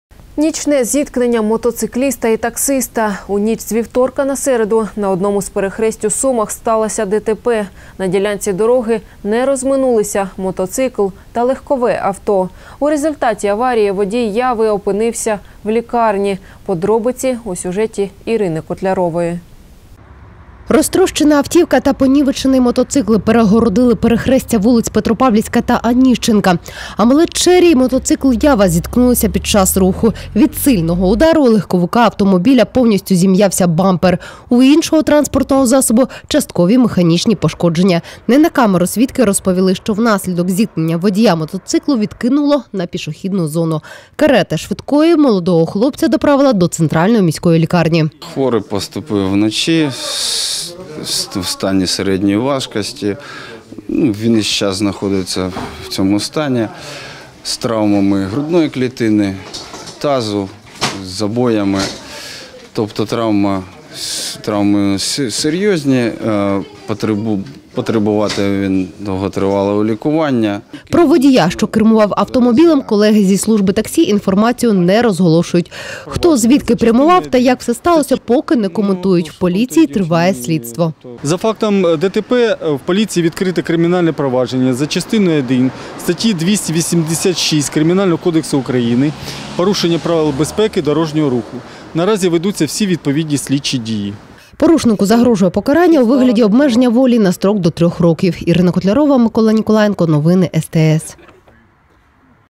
В Сумах водії не поділили перехрестя
Нічне зіткнення мотоцикліста і таксиста. В ніч з вівторка на середу на одному з перехресть у Сумах сталася ДТП: на ділянці дороги не розминулися мотоцикл та легкове авто. В результаті аварії водій «Яви» опинився у лікарні. Подробиці – у сюжеті Ірини Котлярової.